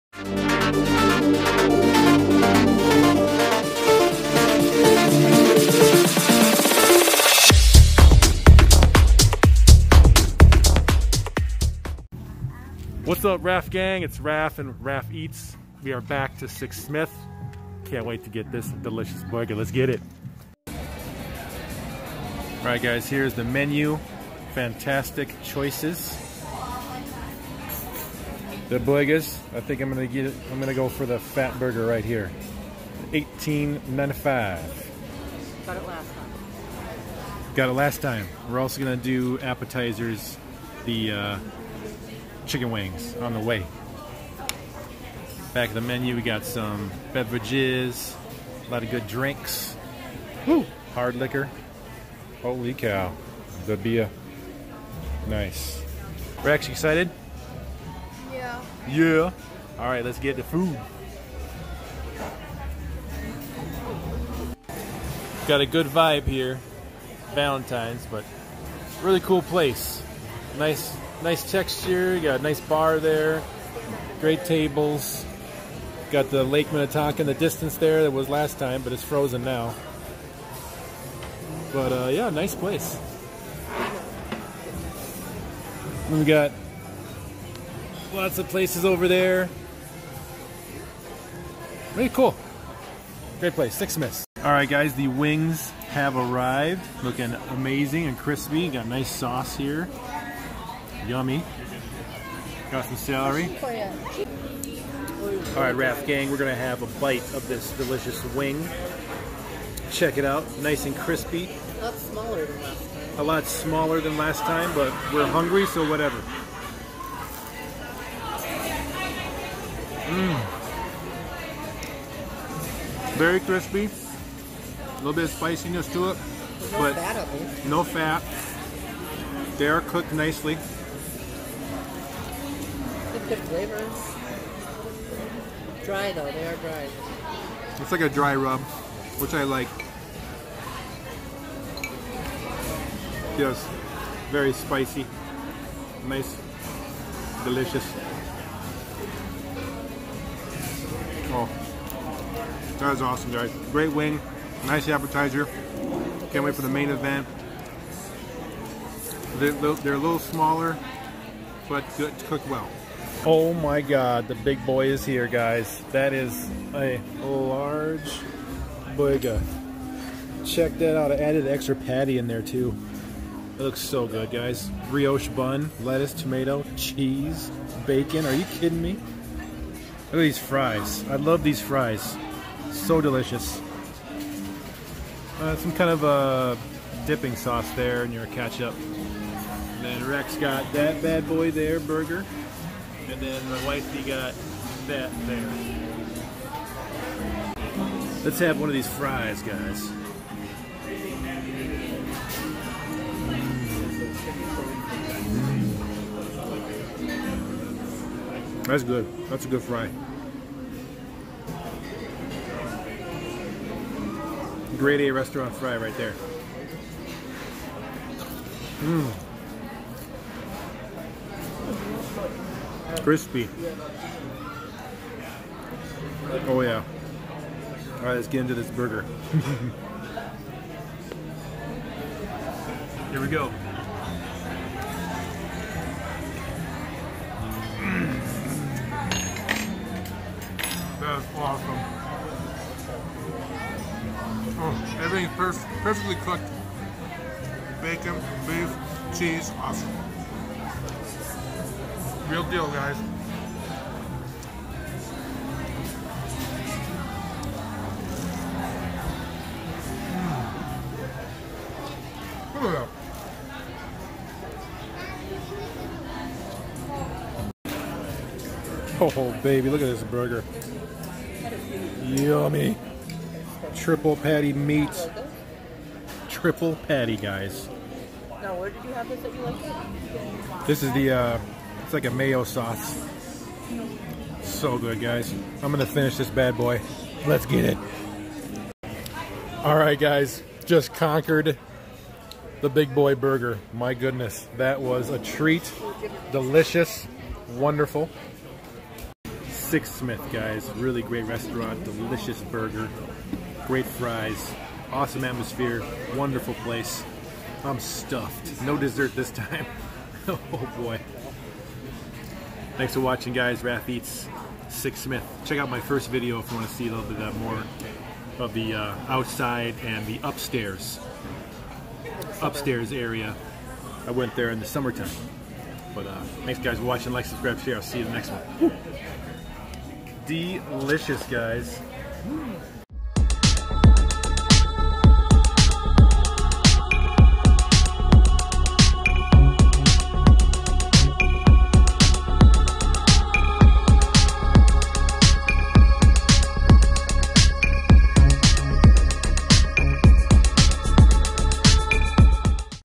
RAF EATS : 6 Smith Restaurant : Big Boy Burger!
Amazing food, great service, awesome restaurant!

Get your Rafcave Gear from the TeeSpring Store -

$ - Super Chats during Live Streams - $